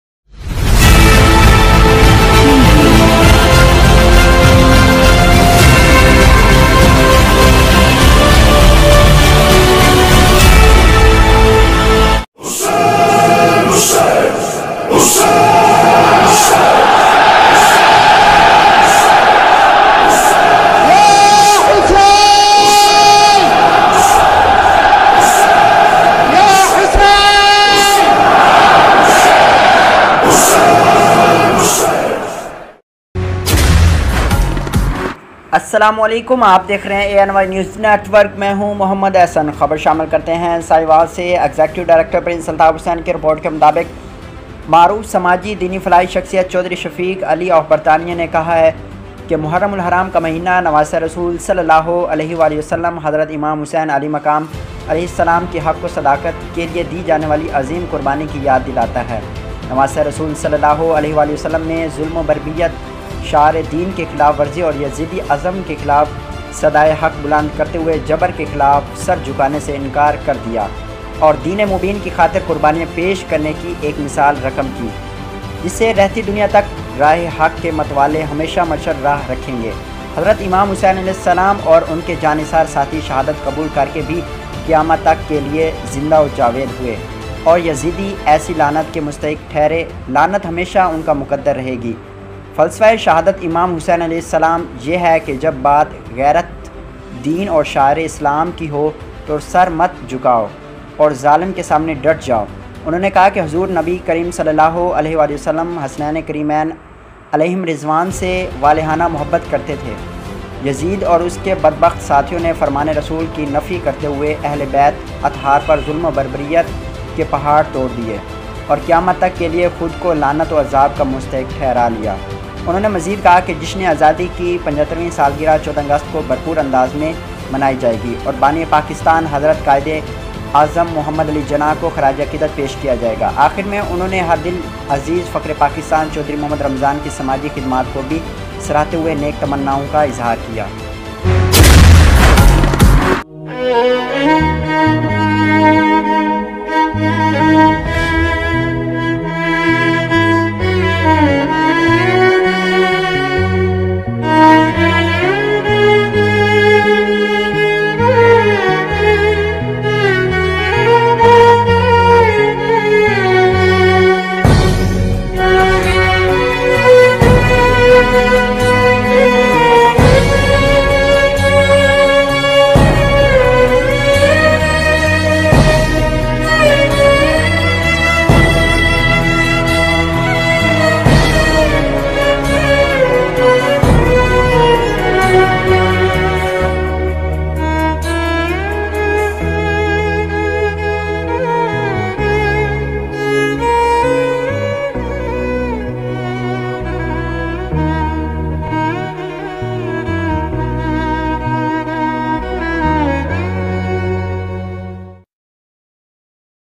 Sahiwal Prince Altaf Hussain Executive Director#daily#any#news#livestream#pakistan
سماجی دینی فلاحی شخصیت چوہدری شفیق علی آف برطانیہ نے کہا ہے کہ محرم الحرام کا مہینہ نواسہ رسول ﷺحضرت امام حسین عالی مقام علیہ السلام کی حق و صداقت کیلئے دی جانے والی عظیم قربانی کی یاد دلاتا ہے نواسہ رسول علیہ السلام نے ظلم و بربریت، شعائر دین کی خلاف ورزی اور یزیدی ازم کے خلاف صدائے حق بلند کرتے ہوئے جبر کے خلاف سر جھکانے سے انکار کر دیا اور دین مبین کی خاطر قربانیاں پیش کرنے کی ایک مثال رقم کی جسے رہتی دنیا تک راہ حق کے متوالے ہمیشہ مشعل راہ رکھیں گے، حضرت امام حسین علیہ السلام اور ان کے جانثار ساتھی شہادت قبول کر کے بھی قیامت تک کیلئے زندہ و جاوید ہوئے اور یزیدی ایسی لعنت کے مستحق ٹھہرے لعنت ہمیشہ انکا مقدر رہے گی، فلسفہ شہادتِ امام حسین علیہ السلام یہ ہے کہ جب بات غیرت دین اور شعائر اسلام کی ہو تو سر مت جھکاؤ اور ظالم کے سامنے ڈٹ جاؤ، انہوں نے کہا کہ حضور نبی اکرم ﷺحسنین کریمین علیھم رضوان سے والہانہ محبت کرتے تھے یزید اور اس کے بدبخت ساتھیوں نے فرمانِ رسول کی نفی کرتے ہوئے اہل بیت اطہار پر ظلم و بربریت کے پہاڑ توڑے اور قیامت تک کیلئے خود کو لعنت و عذاب کا مستحق ٹھہرا لیا۔ انہوں نے مزید کہا کہ جشن آزادی کی 75ویں سالگرہ 14اگست کو بھرپور انداز میں منائی جائے گی اور بانی پاکستان حضرت قائداعظم محمدعلی جناح کو خراج عقیدت پیش کیا جائے گا ۔ آخر میں انہوں نے ہر دلعزیز فخر پاکستان چوہدری محمد رمضان کی سماجی خدمات کو بھی سراہتے ہوئے نیک تمناوں کااظہار کیا۔